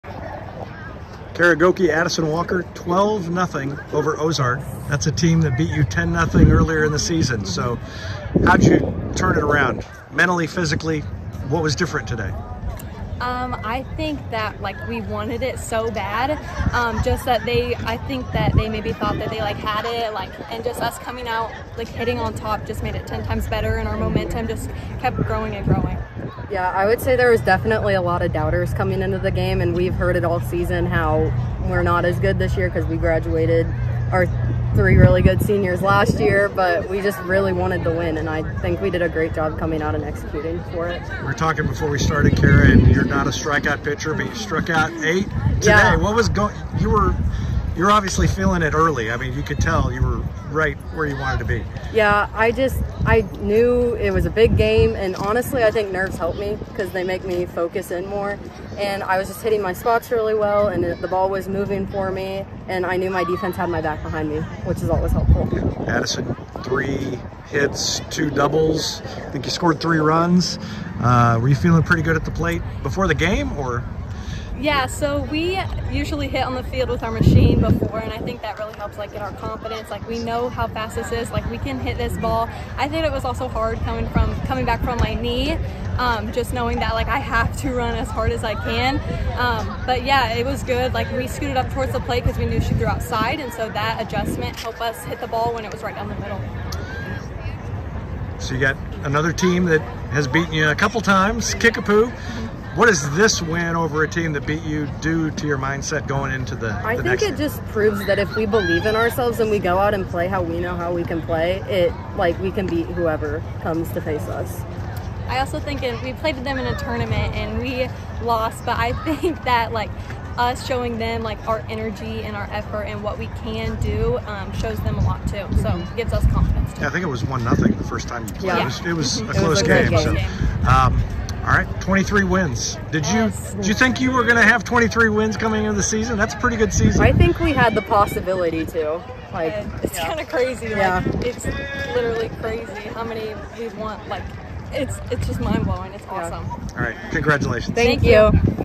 Postgame Show - District Softball vs Ozark
How about a 22-run turnaround? Republic beat Ozark 12-0 Saturday in the district semifinals, just weeks after losing to Ozark 10-0. We talked to winning pitcher Kara Goeke and shortstop Addison Walker about it on our postgame show, presented by State Farm agent Kris Yocum.